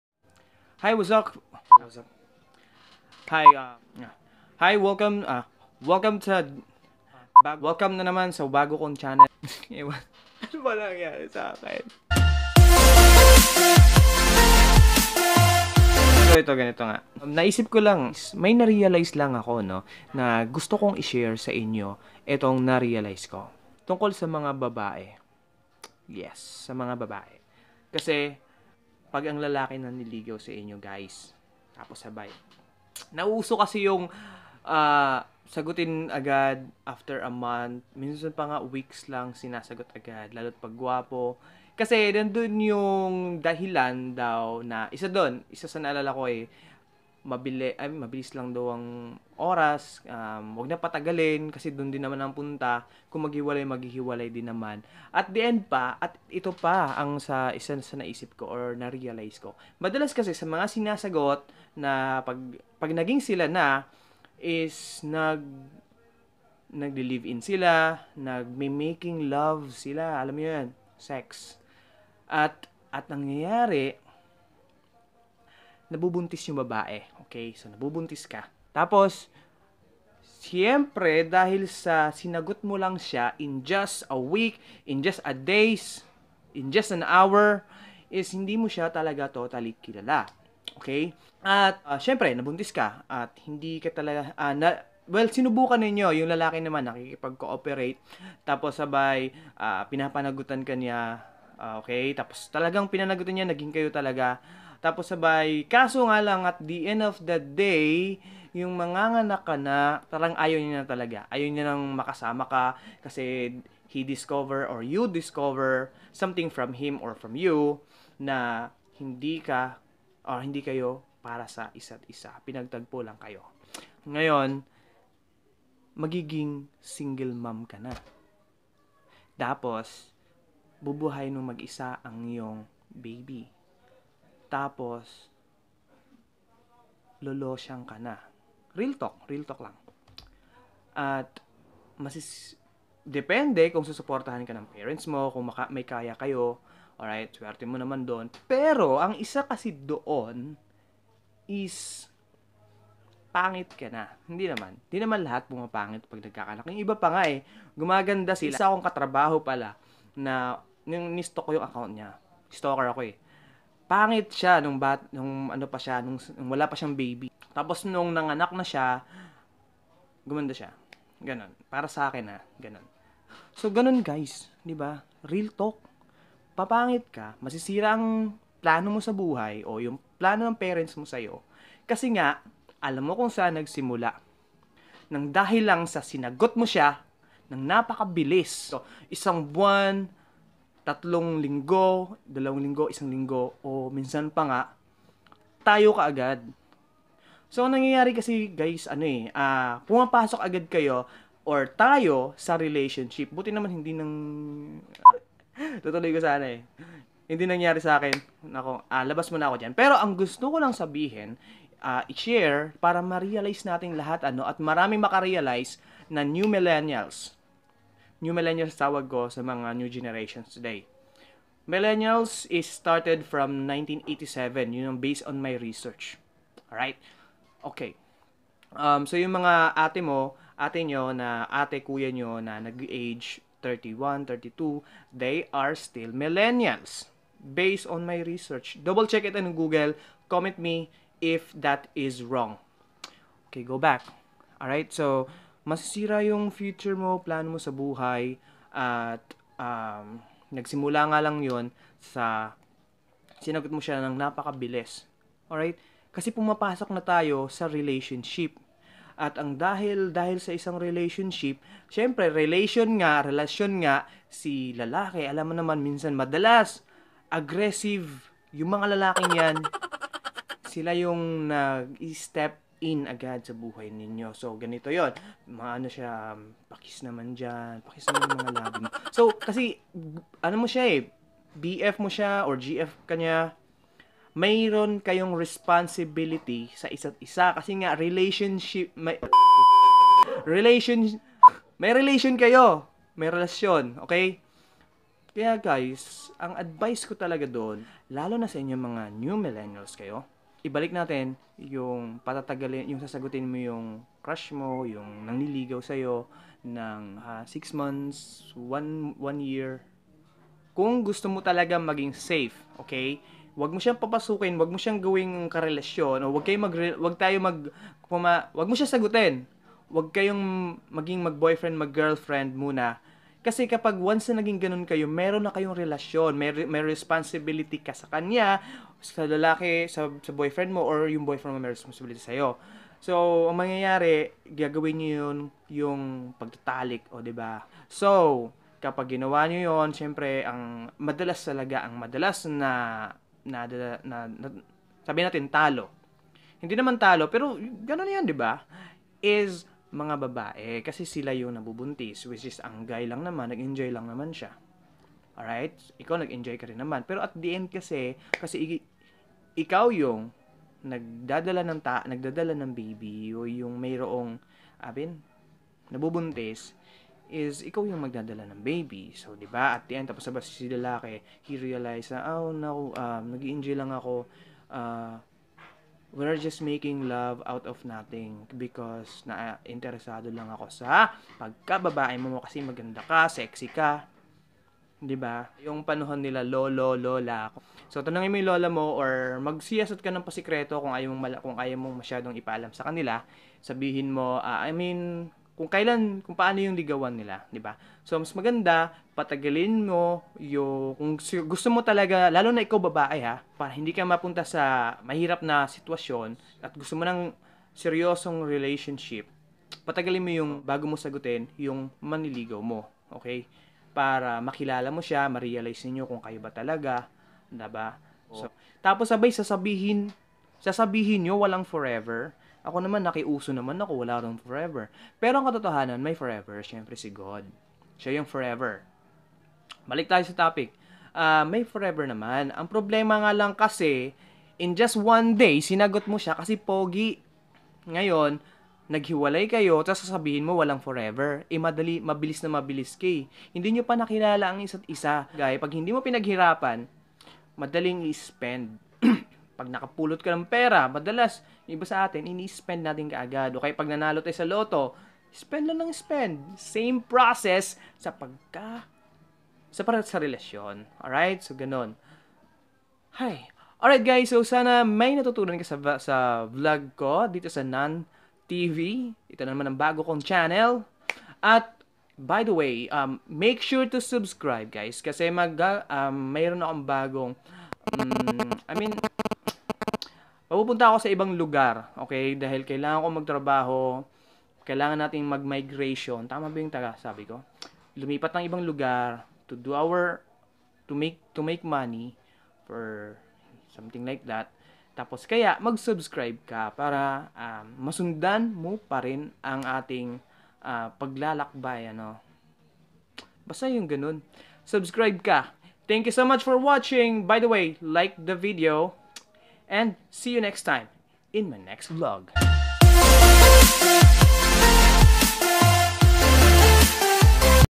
Jowa kaagad wala ng ligaw ligaw pa
Mabilis na pag sagot sa magiging jowa mo ay maaaring isa trahedya sayo